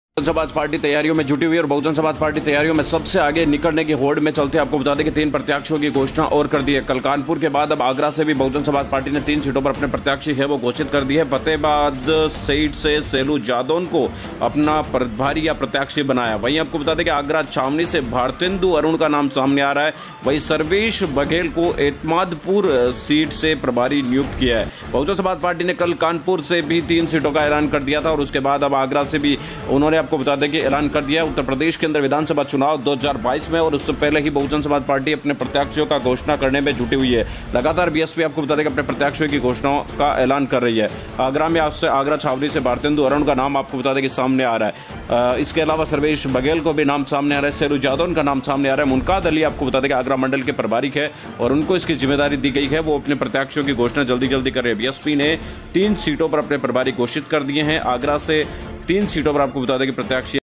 UP Election: बसपा ने आगरा जिले से 3 सीट पर अपने प्रत्याशी घोषित कर दिए गए है | BSP | Mayawati
हमसे जुड़े -